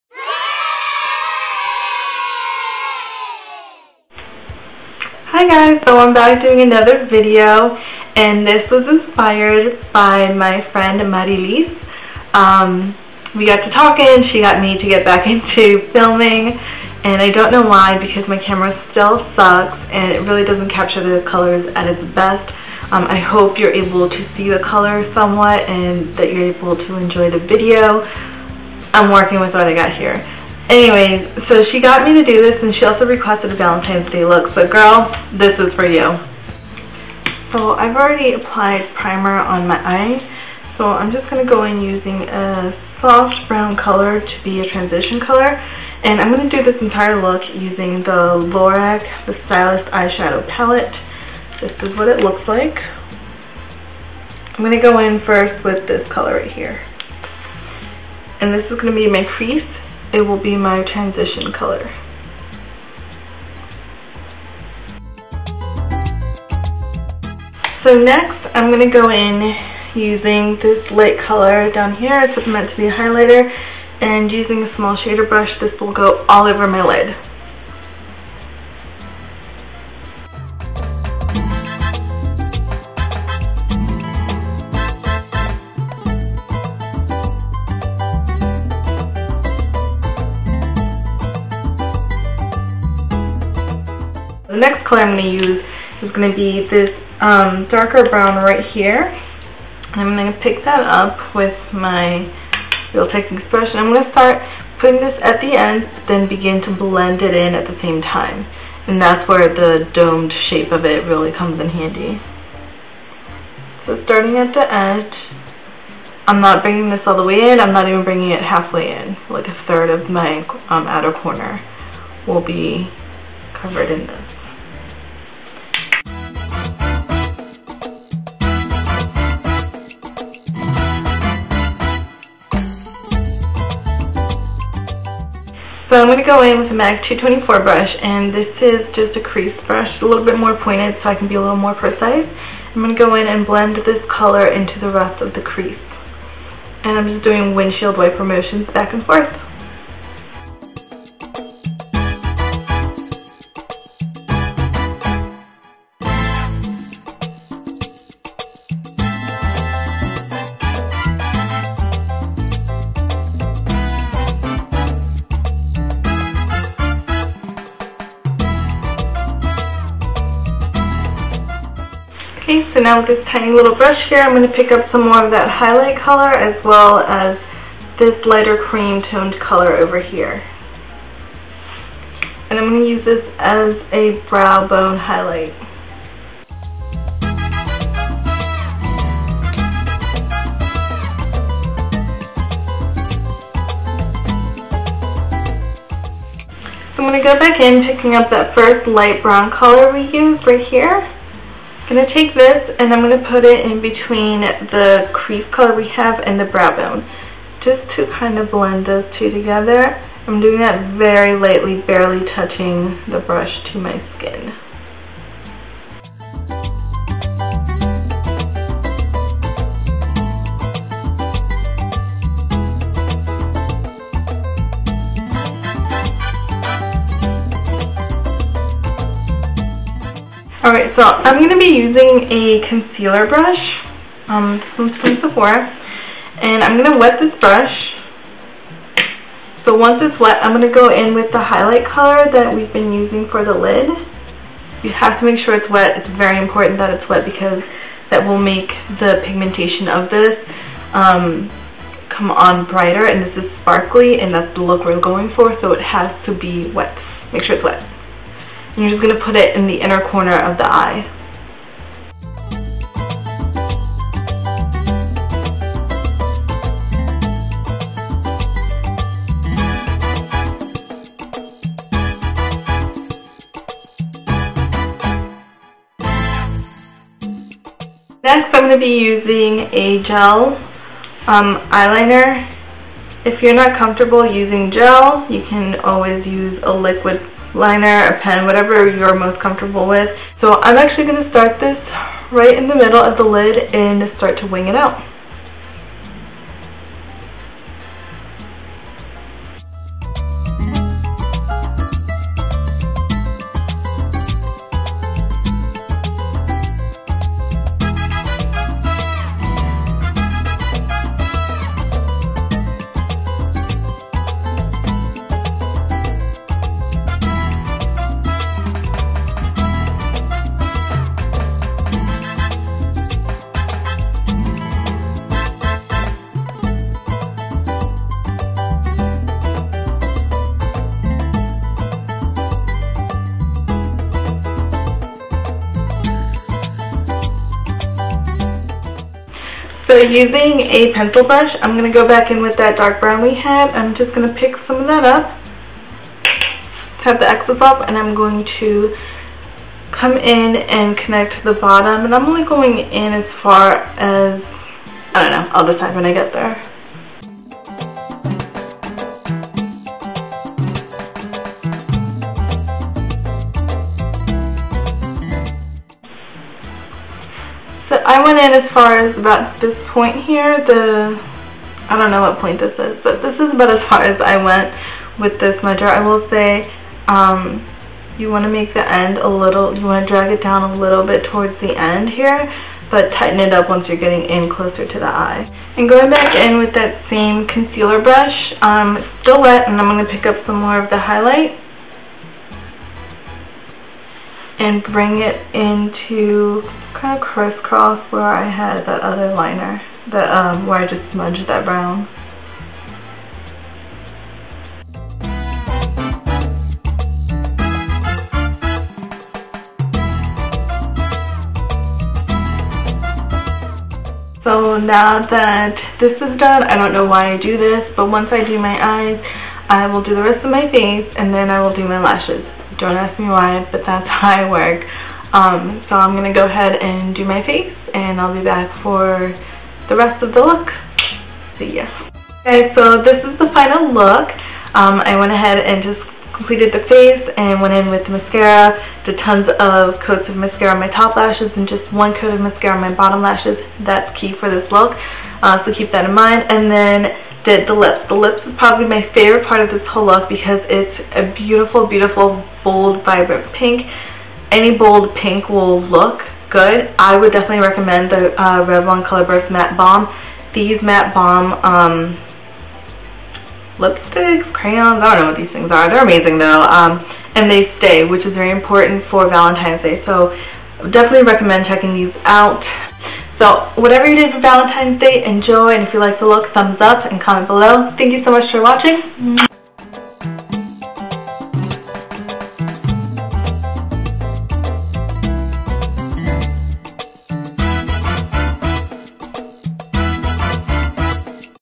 Lorac's The Stylist Palette Valentines Day Look
This Valentine's Day Look was requested and inspired by the lovely Marie Liz. This one's for you!

For the Eyes:
UrbanDecay  Primer Potion (not shown)
Lorac - The Stylist Palette
Lancome - Hypnose Drama Mascara
E.L.F. - Black gel eye liner

Lips:
Revlon - Colorburst Matte Balm "Unapologetic"
Dermstore - Lip Quench

Face:
Benefit - POREfessional primer
   -Maybelline - True Match Lumi Foundation
   -Tarte - 12 hour Amazonian Clay Foundation
   -NYX - Born to Glow Liquid Illuminator in Gleam
Maybelline - Dream Lumi Highlighting Concealer
NARS - Deep Throat
MAC - Soft & Gentle Highlight